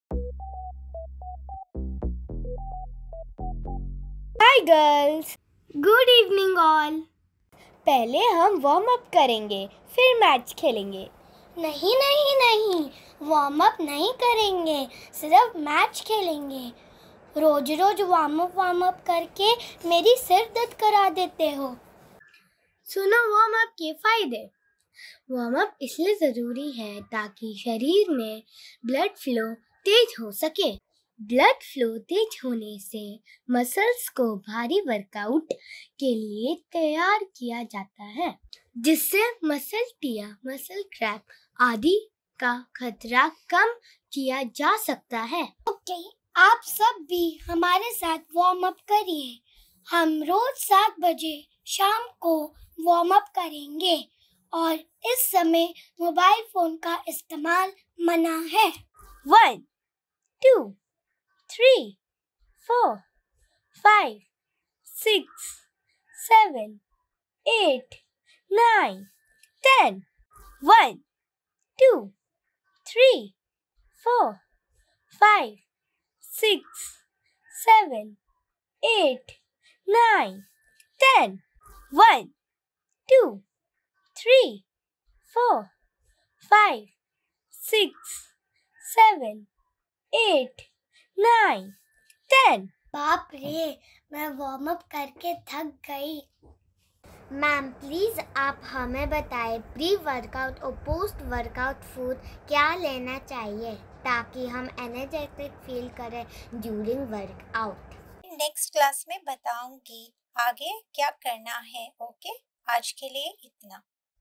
WARM UP BENEFITS
Warming up prevents injuries by loosening your joints, and improving blood flow to your muscles — making your muscles less likely to rip, tear, or twist in a harmful way during your workout